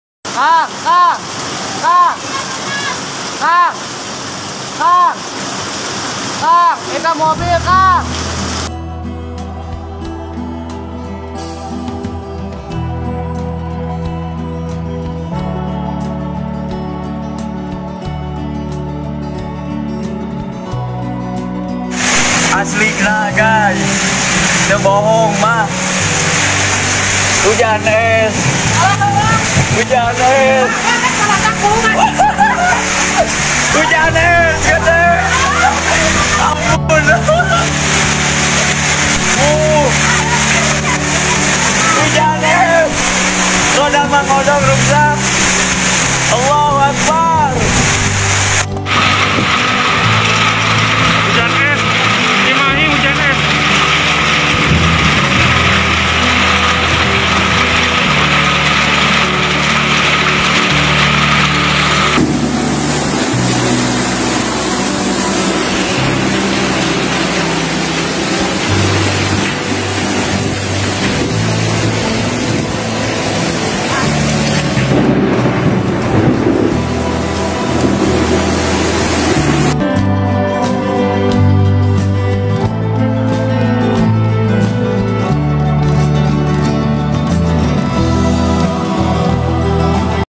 Hujan Es & Mobil Hanyut di Cimahi, Kamis 01/07/21
Hari ini, Kamis 01 Juli 2021 Kota Cimahi di guyur hujan yg sangat deras disertai dengan adanya es.  Menurut BMKG, bahwa hujan es ialah alamiah karena termasuk dalam cuaca ekstrim.
Adapun kejadian hujan lebat disertai kilat atau petir, dan angin kencang berdurasi singkat, lebih banyak terjadi di masa transisi atau musim pancaroba dari musim kemarau atau sebaliknya musim penghujan ke musim kemarau. Fenomena hujan es Cimahi, karena awan cumulonimbus (CB) pada awan ini terdapat 3 partikel : butir air, butir air super dingin dan partikel es.
Selain menyebabkan hujan es, ada juga sebuah mobil hanyut terbawa derasnya hujan sehingga menyebabkan pinggir aliran sungai longsor menyebabkan mobil terbawa arus deras. Lokasinya di komplek Mekarwangi/Saibi pinggiran sungai Cikendal.
Source: @fbinfoCimahi.co